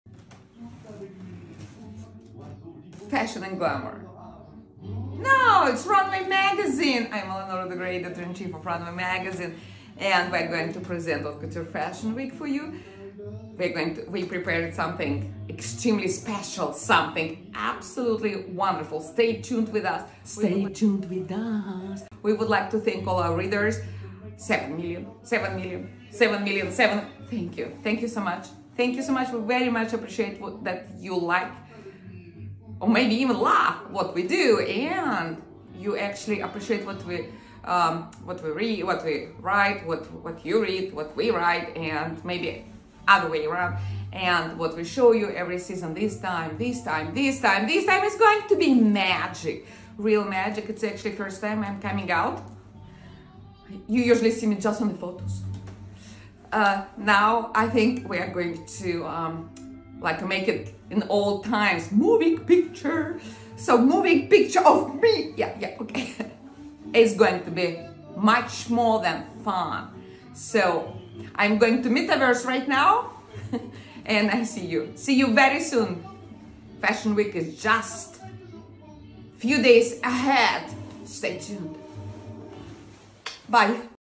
Eleonora de Gray - Editor in Chief of RUNWAY MAGAZINE announcing launch of RUNWAY WEB3
Eleonora de Gray - Editor in Chief of RUNWAY MAGAZINE announcing launch of RUNWAY WEB3 / RUNWAY METAVERSE MAGAZINE

First Metaverse Magazine idea belongs to Eleonora de Gray, Editor-in-Chief of RUNWAY MAGAZINE. Web3 magazine is offering to the readers new interactive experience based on new technologies Web3. This project launched in July 2, 2022 exclusively for Runway Magazine readers (Date when the service was first rendered under the trademark - First Use In Commerce). And public launch dates September 20, 2022.